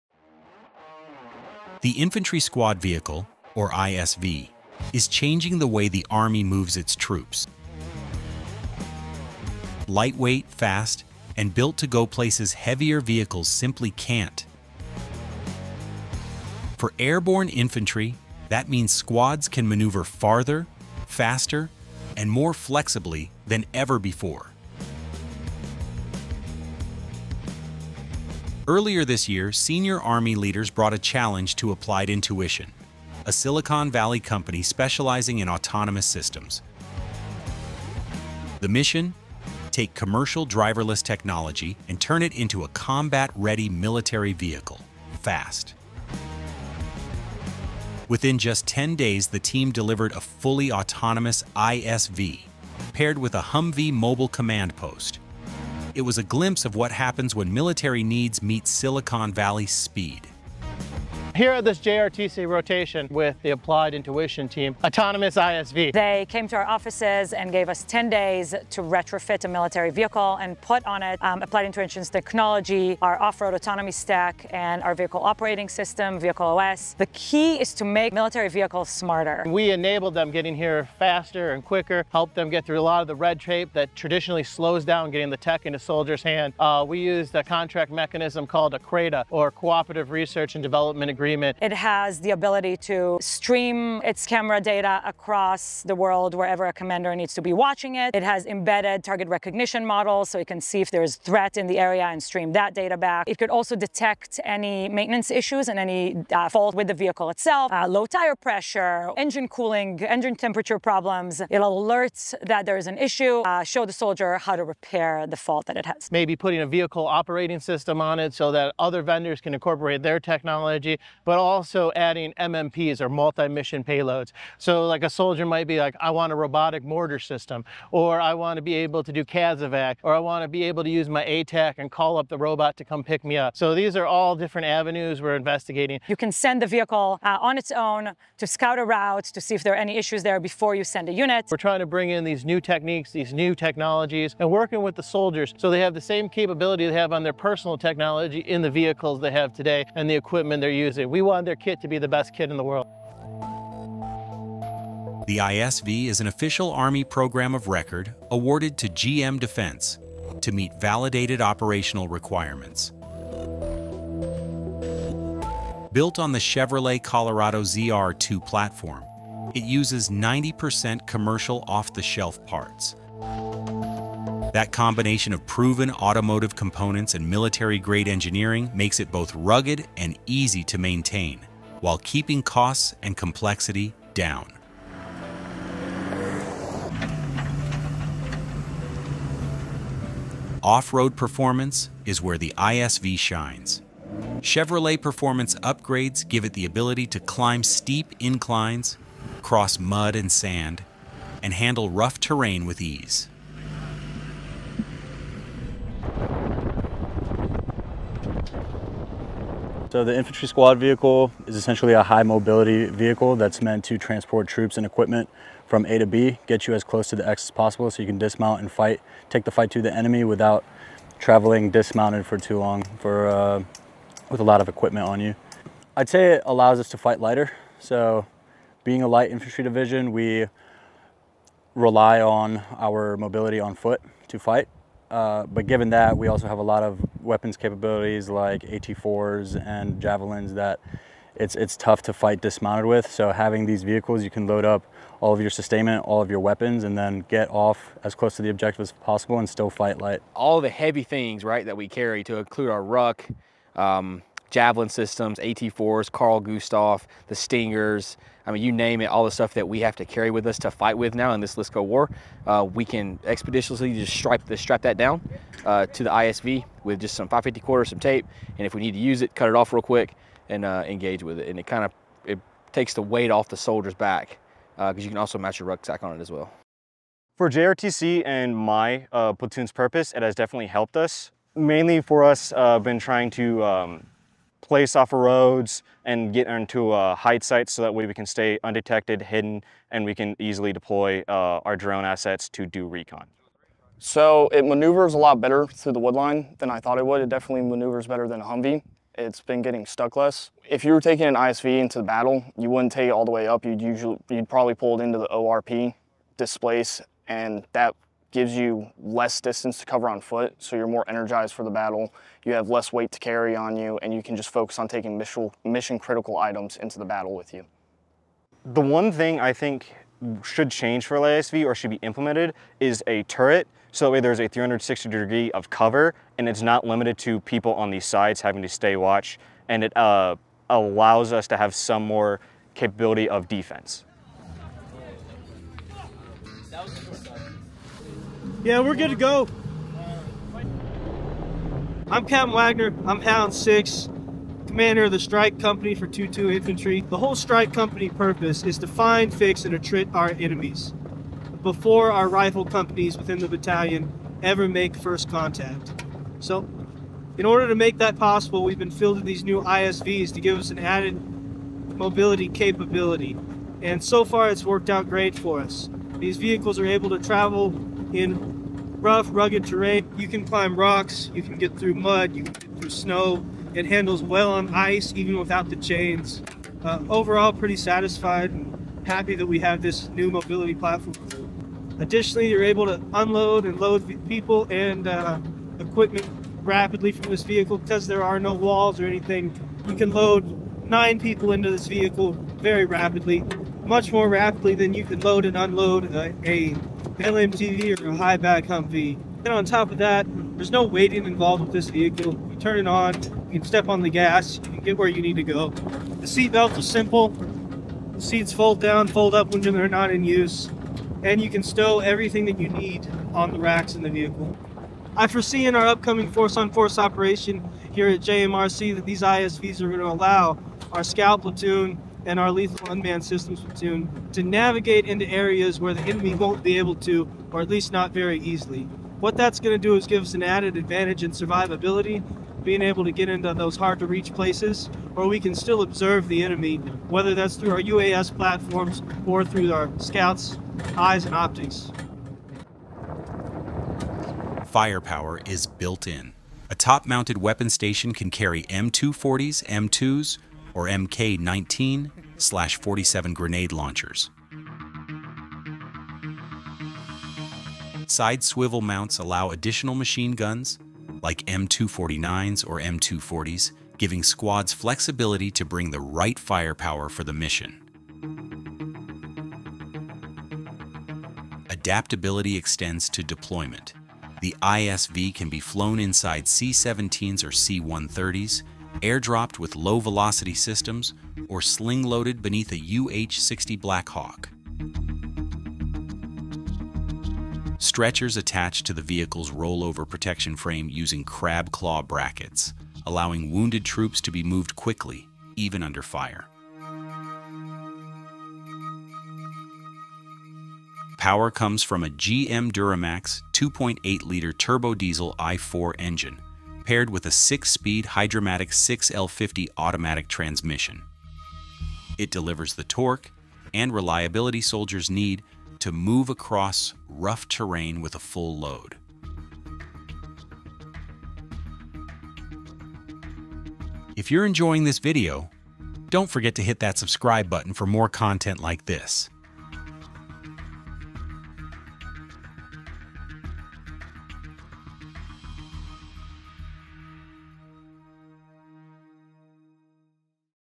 The Army’s 9-Seater ISV Could Transform How Soldiers Go to Battle
The U.S. Army’s Infantry Squad Vehicle, or ISV, is a lightweight 9-seat transport designed to move troops quickly across rough terrain where heavier vehicles can’t operate. Built by GM Defense on the Chevrolet Colorado ZR2 platform, it uses about 90% commercial off-the-shelf parts, making it easier and cheaper to maintain. The ISV can be carried inside a CH-47 Chinook, sling-loaded under a UH-60 Black Hawk, or airdropped from a C-130 or C-17. Although it isn’t armored, the Army is testing autonomous and semi-autonomous systems to allow the ISV to navigate without a driver, potentially transforming how infantry units are deployed in future battles.